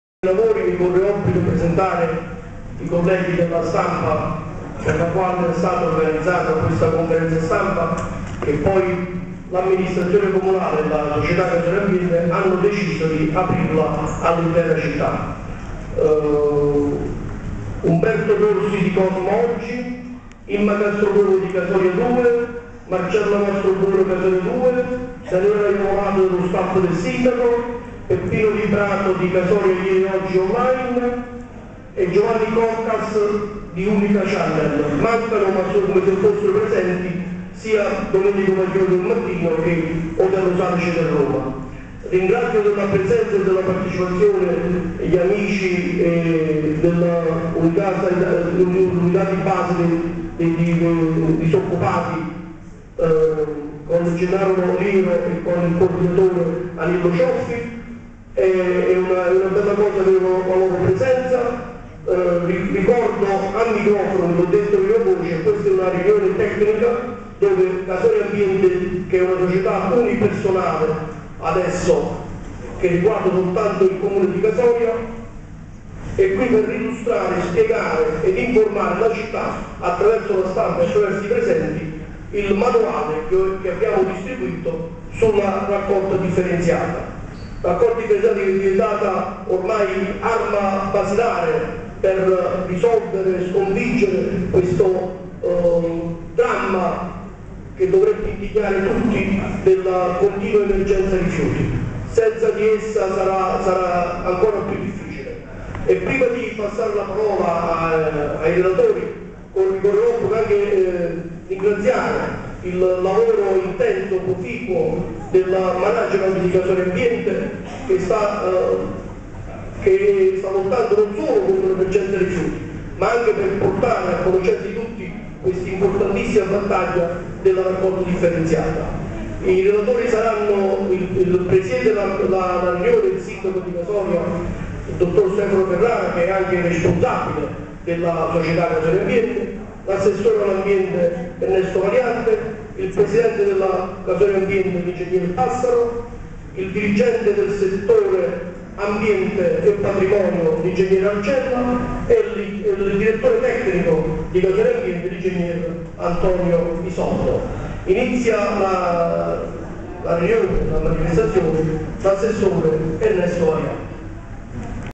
Casoria 11/01/2011 - Conferenza stampa "Casoria Ambiente" (01)
"Casoria Ieri & Oggi online" presenta:
Casoria martedì 11 gennaio 2011 presso la sala del Consiglio Comunale.
"La raccolta differenziata è il futuro".
Presentato il nuovo manuale per l'utenza presso la Sala Consiliare di Piazza Cirillo, prodotto dalla società Casoria Ambiente S.p.A., per la raccolta differenziata (edizione Gennaio 2011).
Apertura dei lavori e introduzione dell'addetto stampa Ferdinando Troise